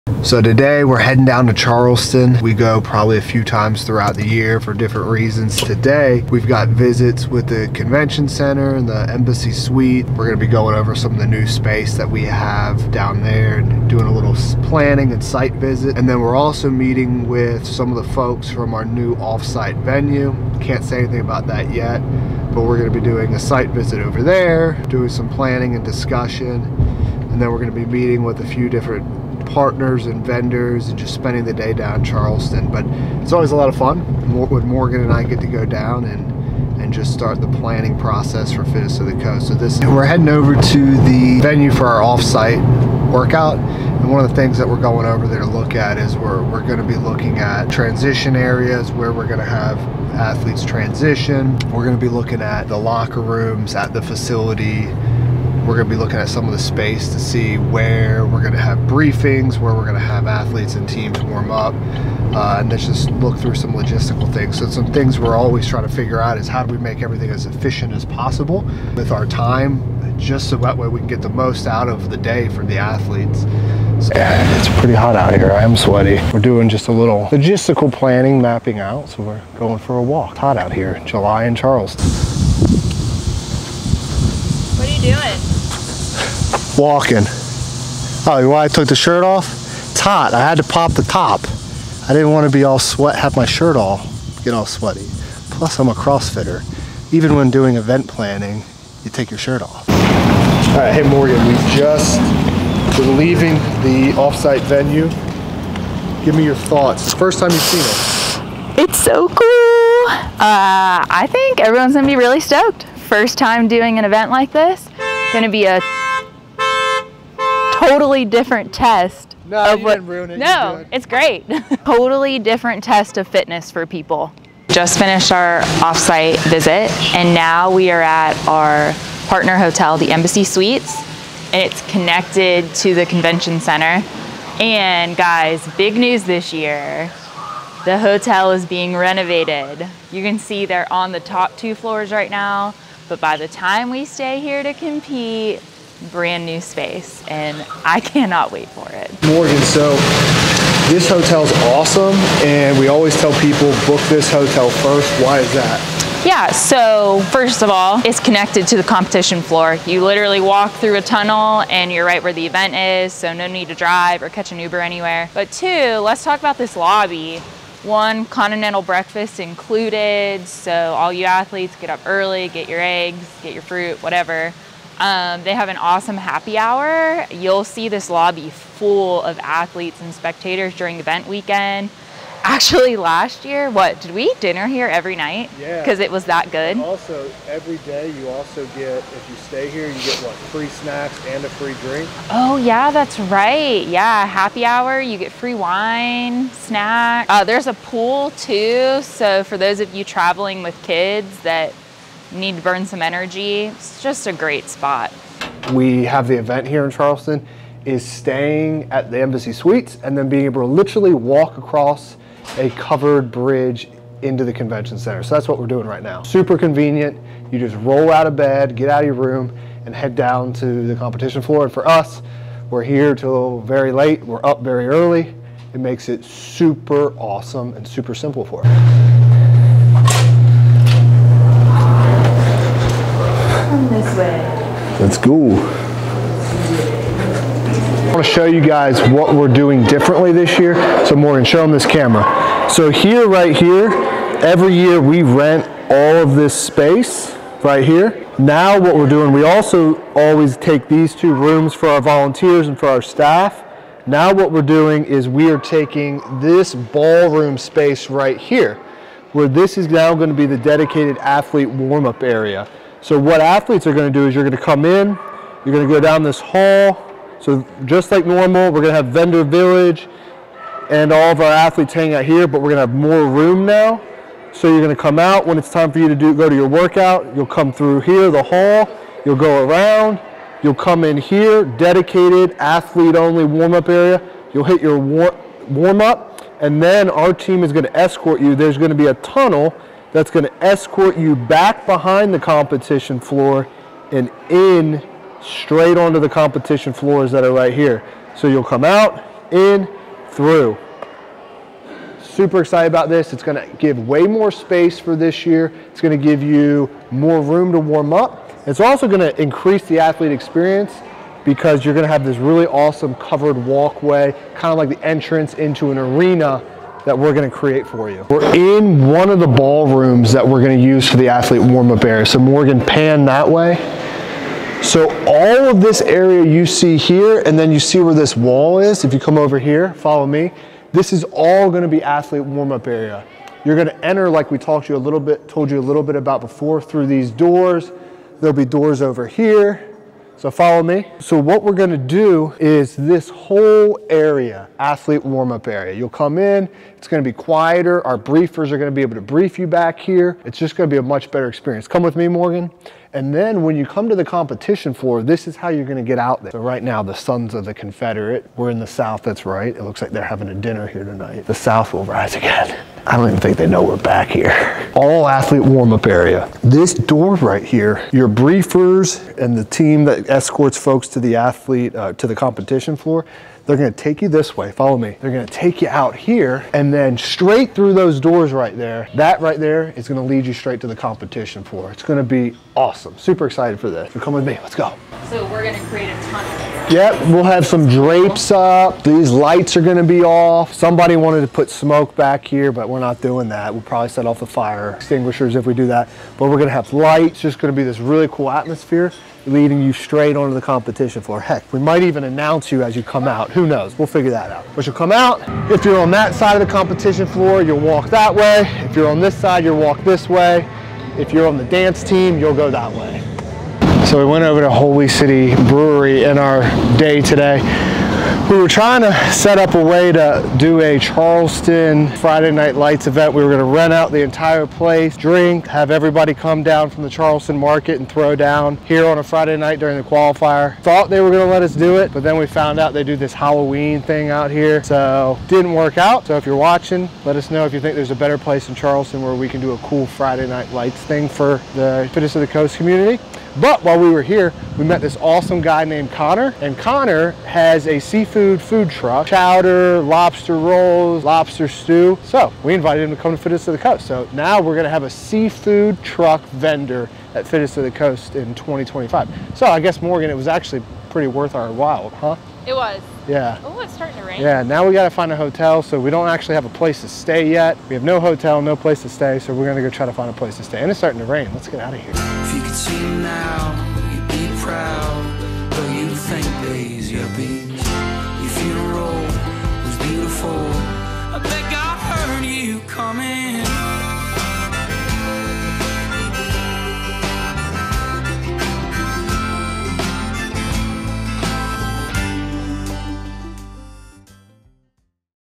We visit the NEW offsite workout venue
Kyle and Morgan Oland from FOTC take you behind the scenes on a site visit to Charleston in preparation for the 2024-25 season.

Fittest of the Coast 2024-25 Competition Season:

Online Qualifier: October 3-29, 2024 (registration opens August 1)

Fittest of the Coast Championship (Charleston, SC): February 7-9, 2025